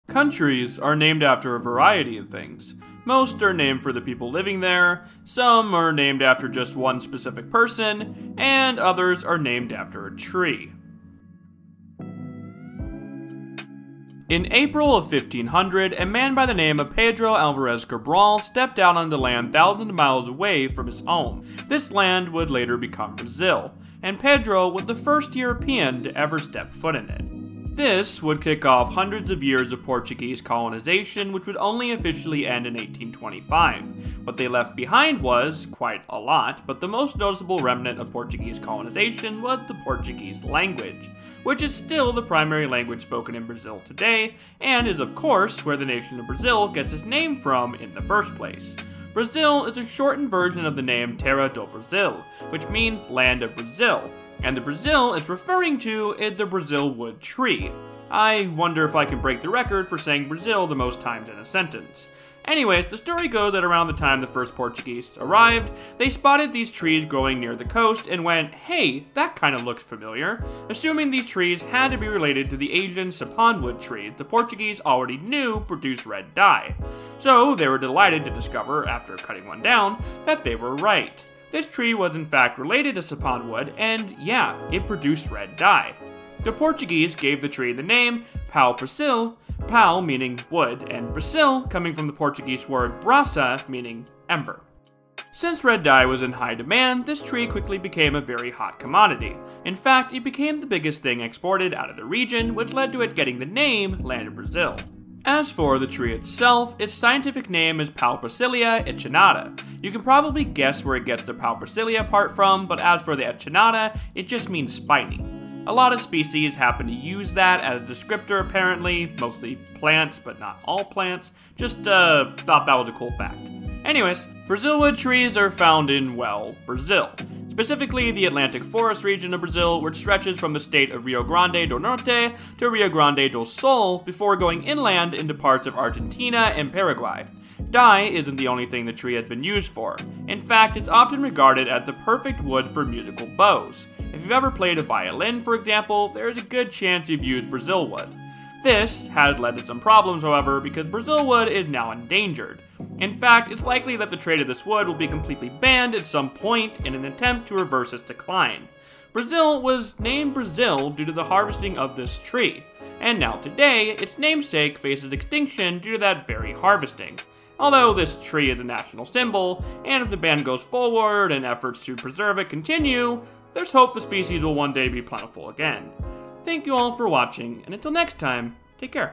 Brazilwood - The Tree That Named A Country
Some countries are named for the people living there, some are named for one specific person, however others...are named after trees.

Additional Credits:

Mauro Halpern (Flickr) for Brazilwood Tree pictures used throughout the video.

NASA and Miguelrangeljr for the map of the Atlanic Forest